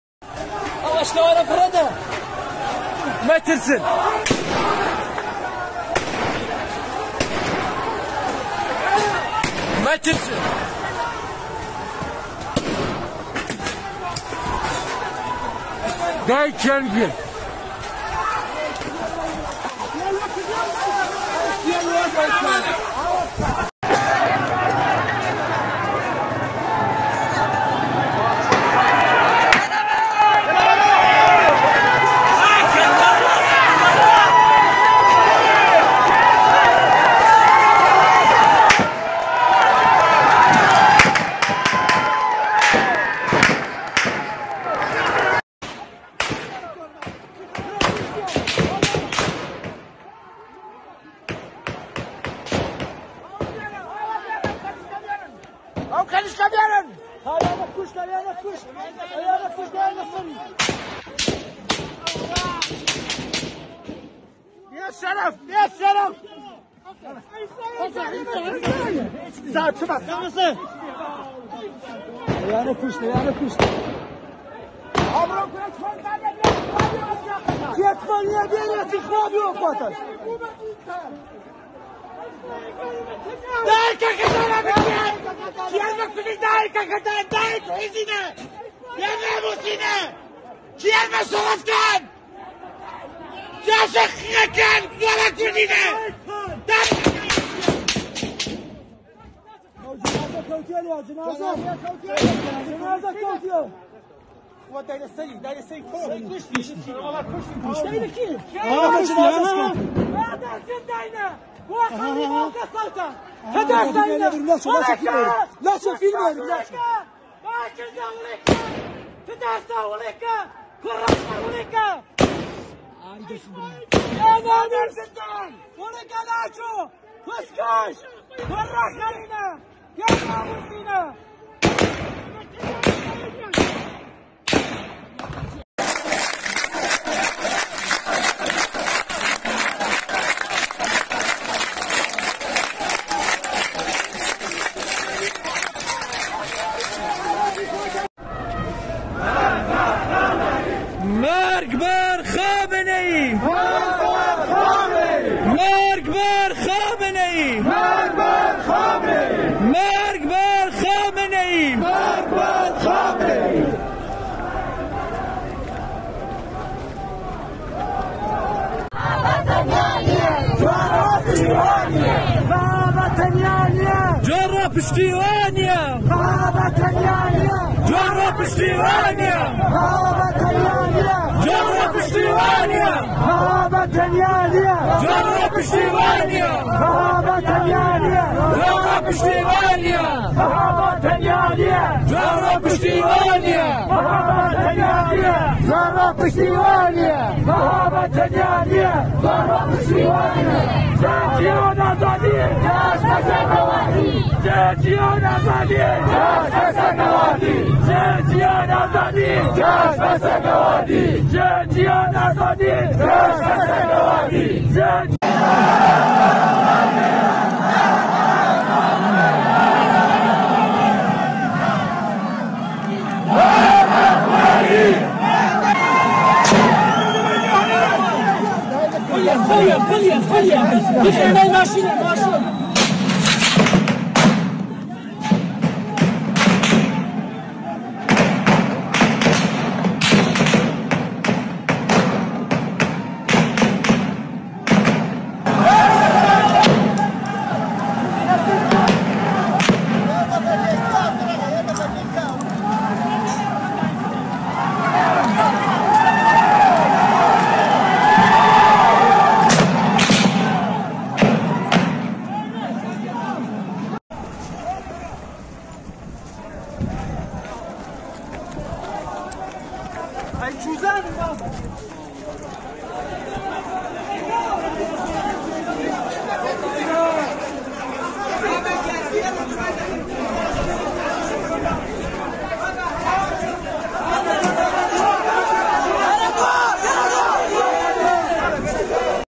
Iran revolution-Islamic regime uses live ammunitions and chemical gases in Javanrud-21Nov.22
Iran revolution
Mahsa Amini
Javanrud